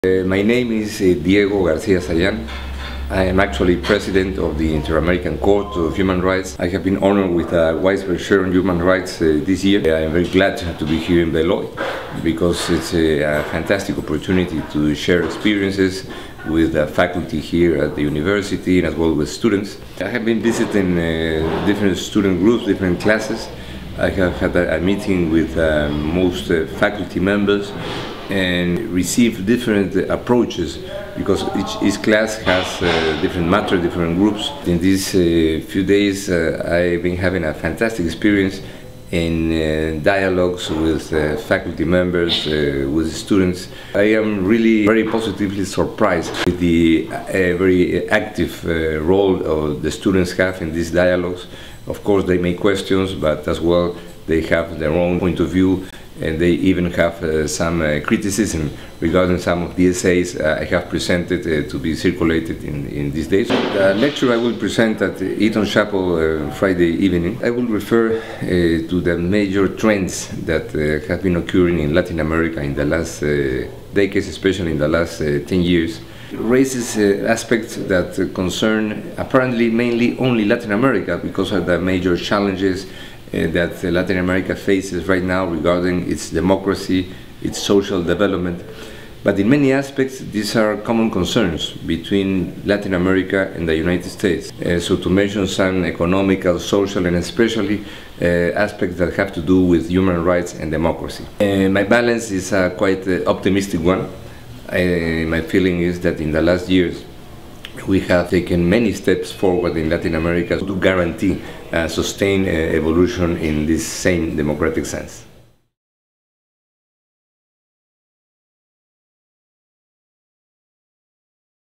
Diego Garcia-Sayan
Weissberg Chair in International Studies Diego Garcia-Sayan talks about his impressions of Beloit College and gives a preview of his lecture taking place Friday, April 12.